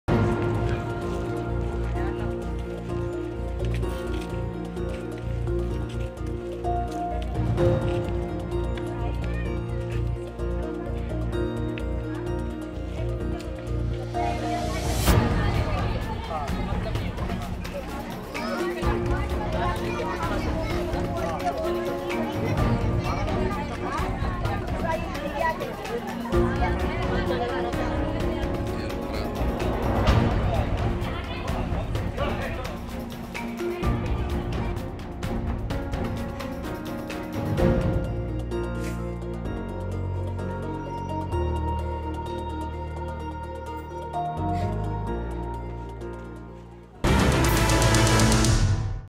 प्यार करने पर लड़की को पंचायत ने सुनाई ऐसी सजा | News Tak
प्यार करने पर लड़की को पंचायत ने सुनाई ऐसी सजा
बिहार के नवादा जिले में एक मुसलमान लड़की को एक हिंदू लड़के से प्यार करना इतना महंगा पड़ गया कि उसके परिवार वालों ने पहले तो उसकी जमकर पिटाई की उसके बाद उसे सजा के तौर पर 5 घंटे तक पेड़ से बांधकर रखा.
girl beaten mother and brother for five hours by tied up with a tree as she escaped with lover in nawada
About News Tak (न्यूज़ तक):